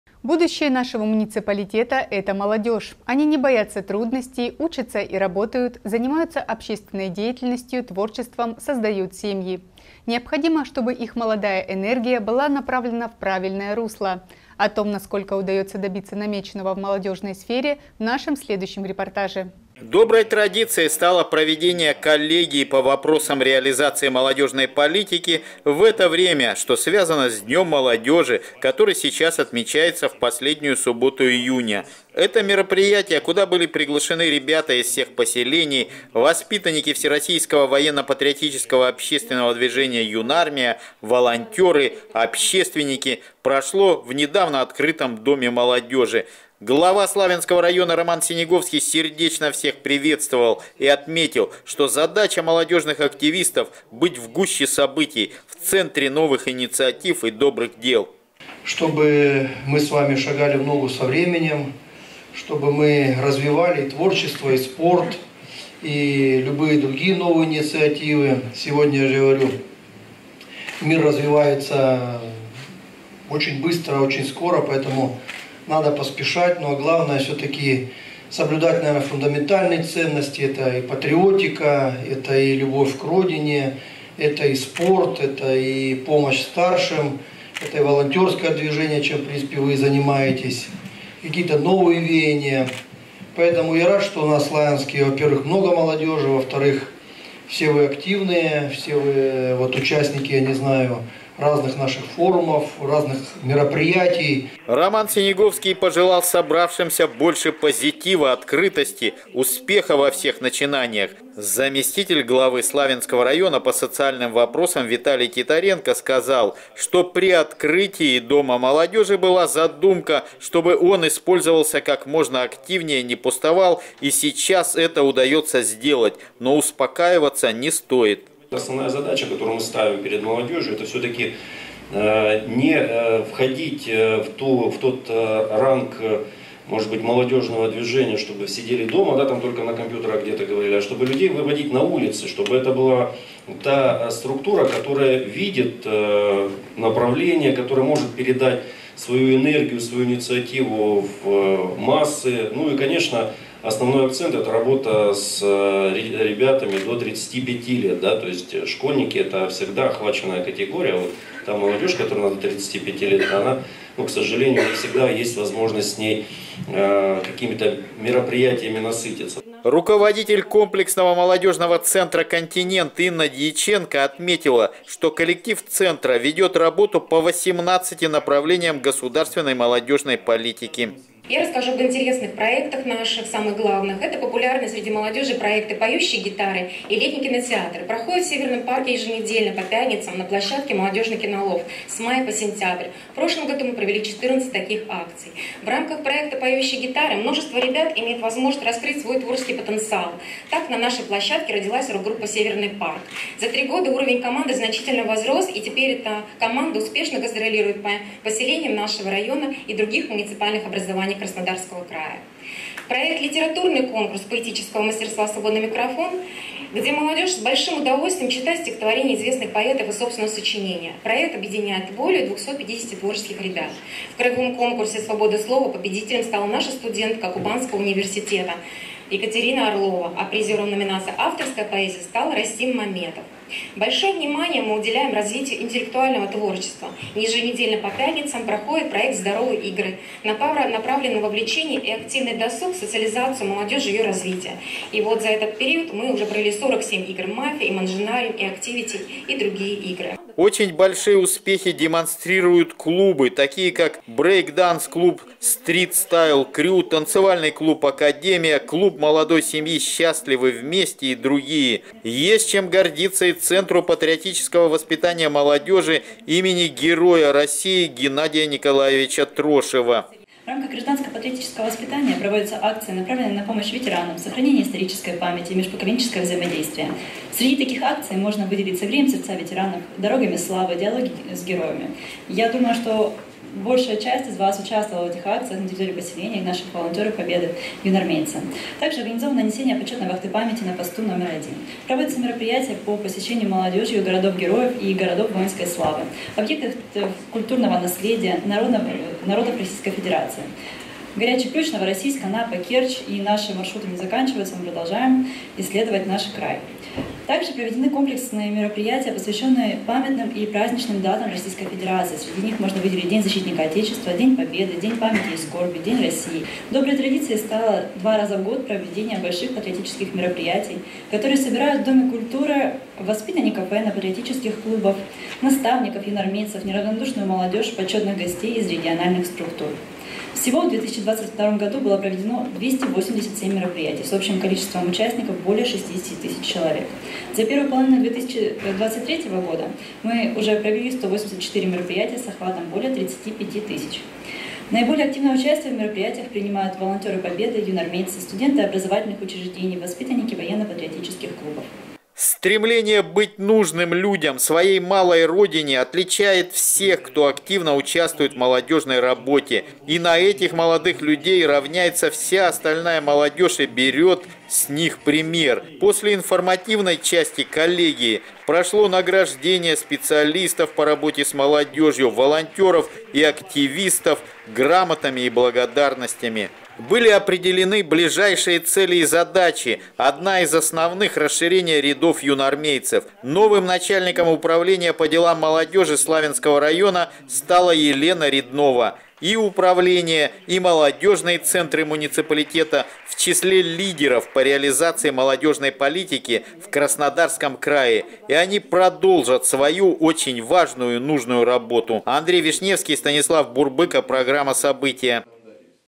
Молодежная политика
Будущее нашего муниципалитета — это молодежь. Они не боятся трудностей, учатся и работают, занимаются общественной деятельностью, творчеством, создают семьи. Необходимо, чтобы их молодая энергия была направлена в правильное русло. О том, насколько удается добиться намеченного в молодежной сфере, в нашем следующем репортаже